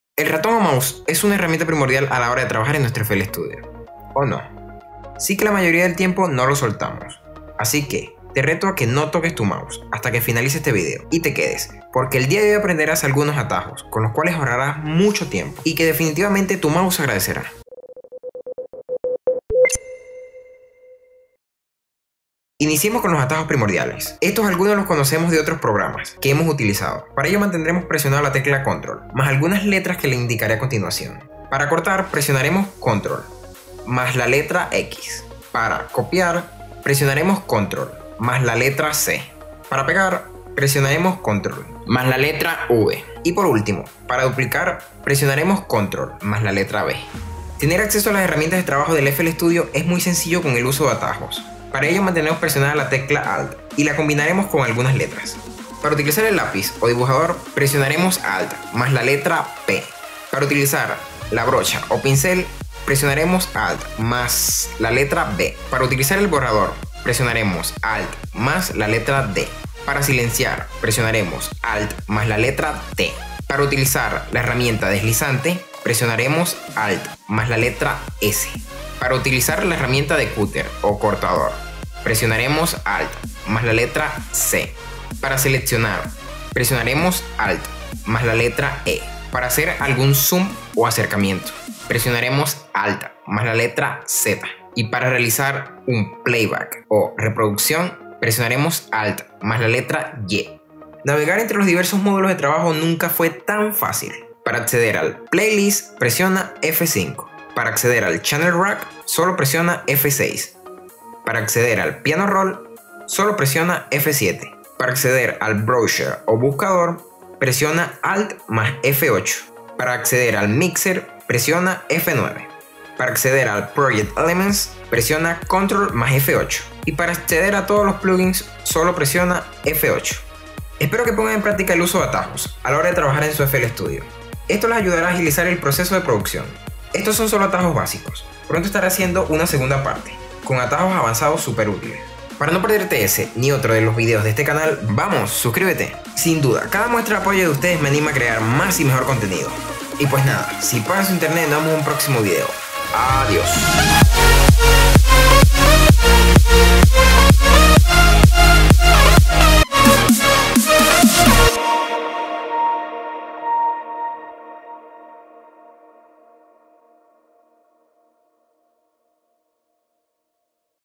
⌨️ FL STUDIO: ATAJOS del TECLADO en FL Studio | ATAJOS que tú MOUSE AGRADECERA
En el video de hoy estaremos aprendiendo *Atajos Básicos* en nuestro FL Studio.

Los cuales sin duda nos ayudarán a trabajar más rápido y que nuestro mouse agradecerá.

*Descarga la guia con todos los atajos vistos en este video*

HEY!

¿Quieres colaborar para la creación de muchos y mejores videos?